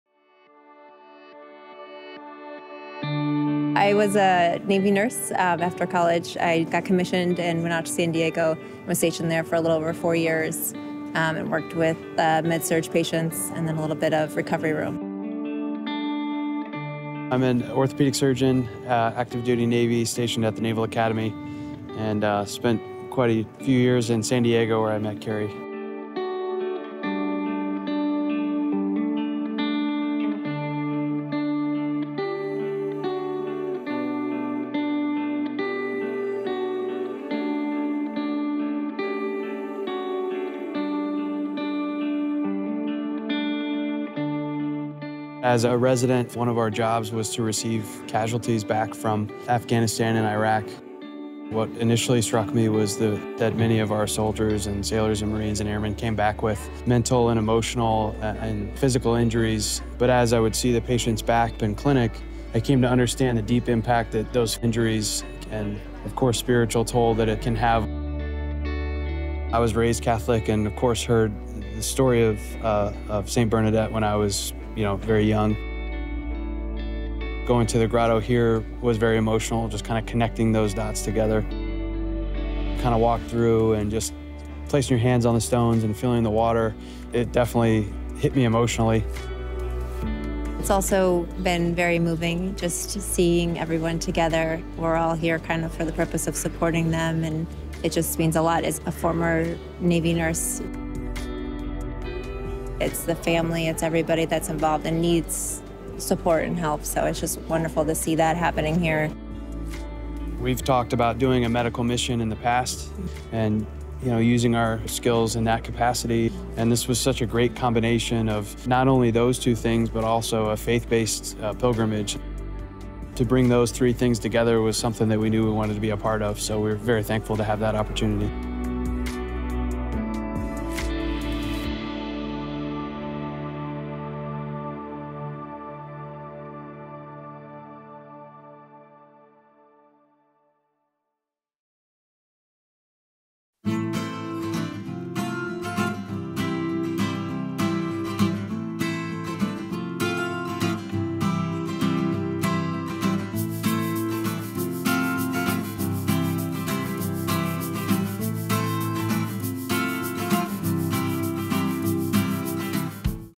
U.S. Navy Couple Help Pilgrims at Lourdes
Lance and Keri LeClere met each other while stationed in San Diego, Calif., as part of their service in the U.S. Navy, with the former as an orthopedic surgeon and the latter as a registered nurse. As part of his residency, Dr. Lance received casualties from Afghanistan and Iraq, while Keri worked with MED search patients and in the recovery room. The pair were searching for an opportunity to participate in a medical mission, ultimately volunteering to help pilgrims in the 2019 Warriors to Lourdes. The trip was a rewarding, faith-fulfilling experience where they witnessed the awesome healing afforded to members of the military in Lourdes.

The Knights of Columbus, in partnership with the Archdiocese for Military Services USA, provides the opportunity and means for our non-wounded, wounded, ill and injured U.S. warriors to participate in the Warriors to Lourdes pilgrimage for a period of prayer and spiritual guidance.